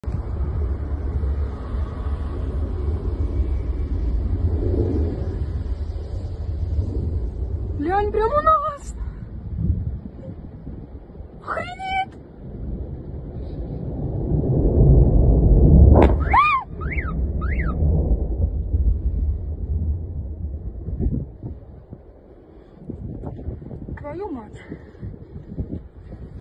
Aereo militare russo precipita a Belgorod
Aereo militare russo precipita a Belgorod. Mosca: “A bordo 65 prigionieri ucraini”. Ma Kiev: “L’abbiamo abbattuto noi, trasportava missili”.